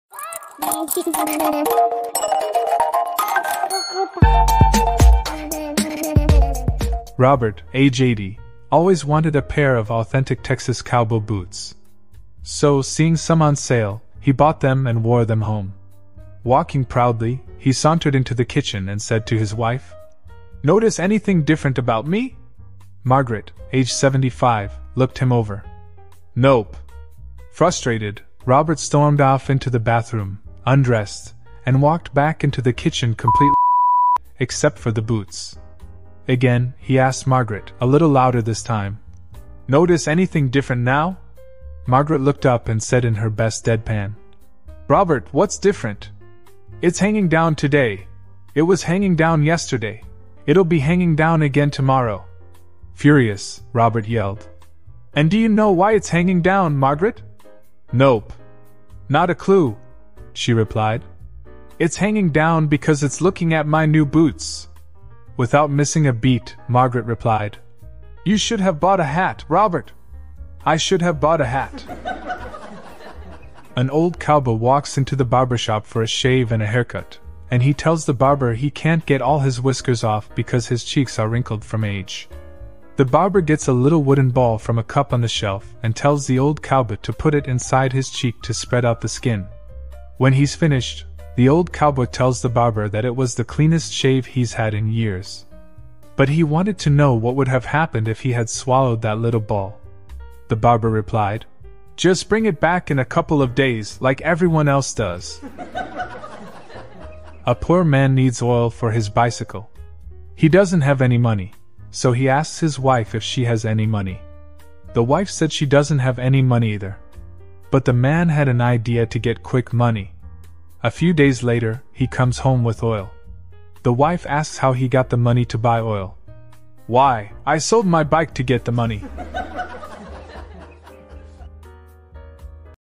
😂 Daily Jokes | So this old man stormed off into the bathroom,.
Joke : 1
So this old man stormed off into the bathroom,

TIMESTPAMPS:

00:06 First joke of the day
01:25 Punchline of the Joke of the day